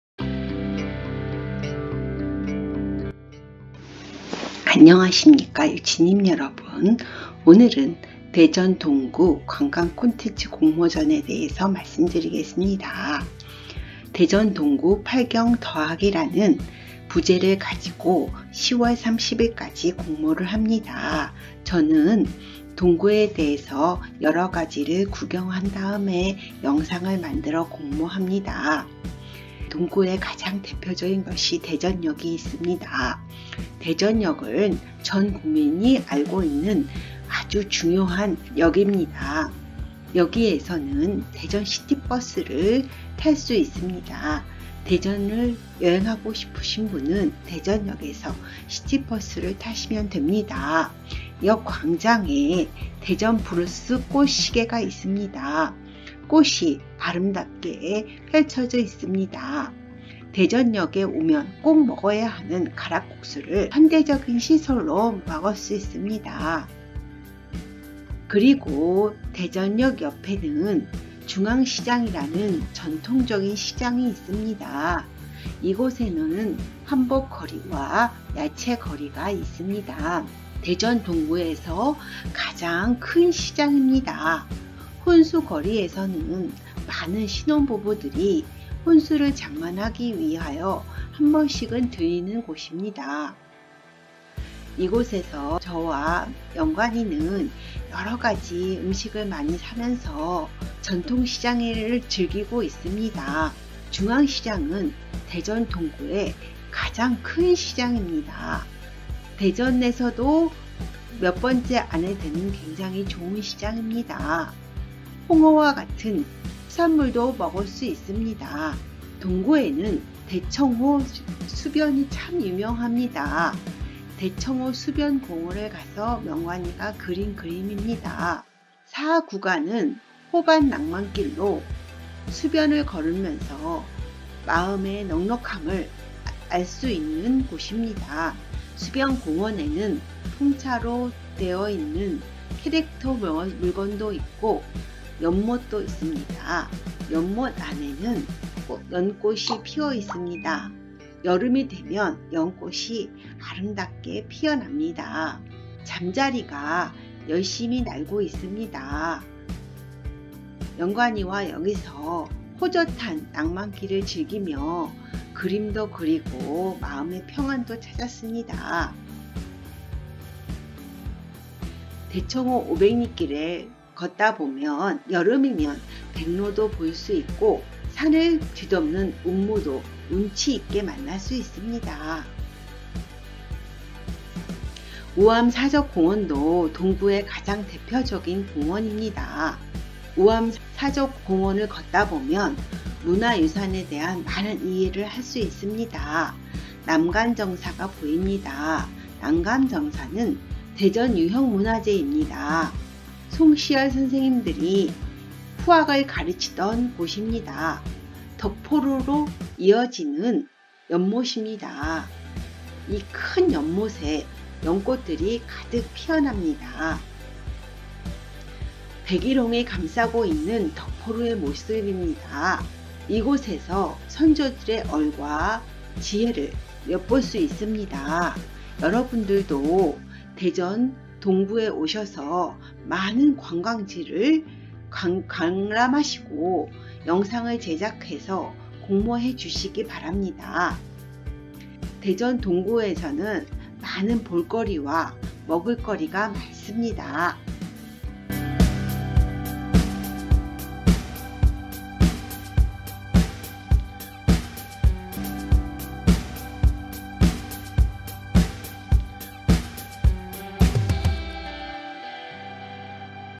대전동구관광콘텐츠공모전
대전동구관광콘텐츠공모전에 참여합니다. 대전에서 동구는 8경을 자랑합니다. 8경은 대전역, 중앙시장, 대청호반, 식장산, 상소동 산림욕장, 우암사적공원, 대동하늘공원, 만인산휴양림입니다.  그 중에 4경을 담아보았습니다. 즐감하시기를 바랍니다. 감사합니다.